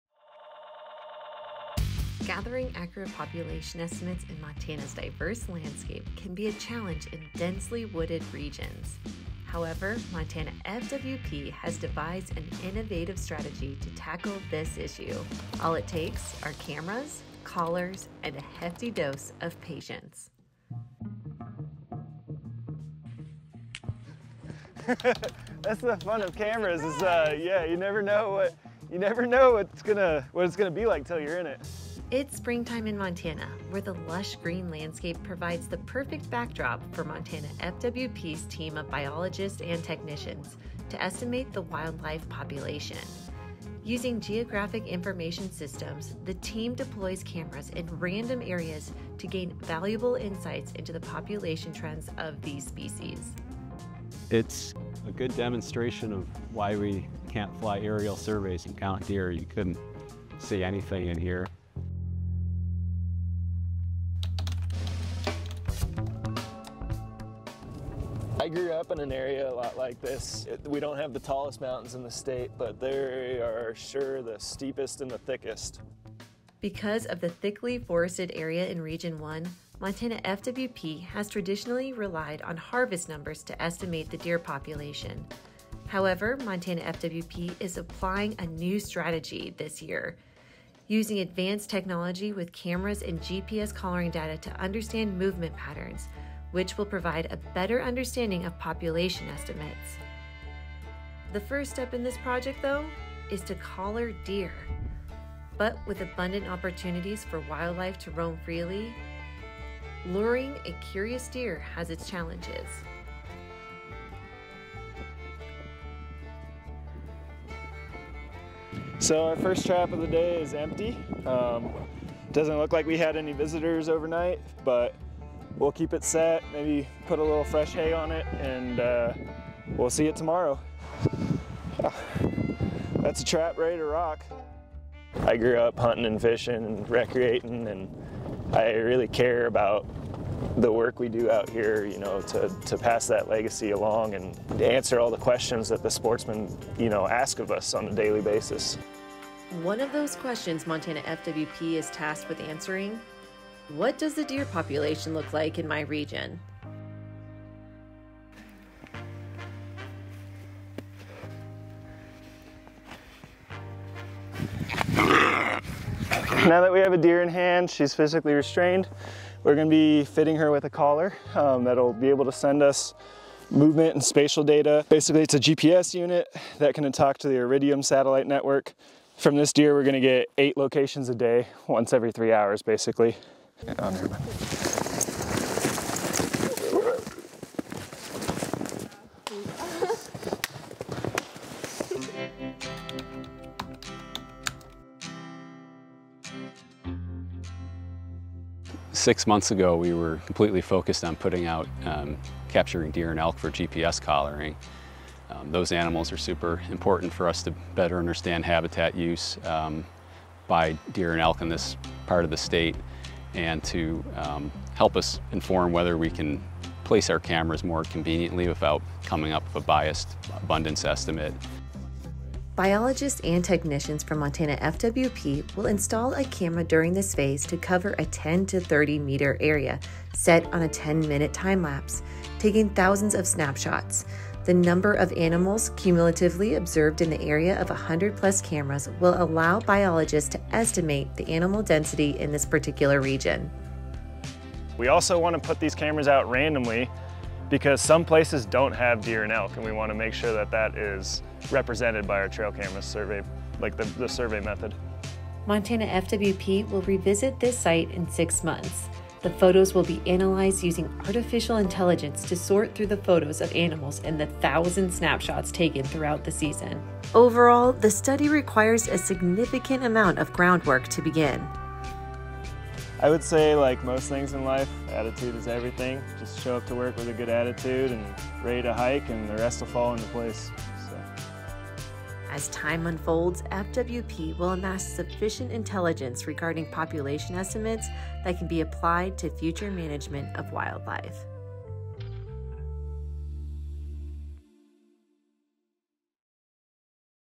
Collar and Camera: New Research on Estimating Population Density
FWP researchers are evaluating the use of remote camera networks as a big game monitoring solution in the heavily forested parts FWP regions 1 and 2 where aerial surveys are often impractical.   If successful, FWP may use this methodology to estimate hunting district big game population sizes on a long-term rotating basis.  Information from this methodology would be used to inform the public of elk, deer and bear population status, inform management decisions, and improve our ability to monitor the effectiveness of management actions.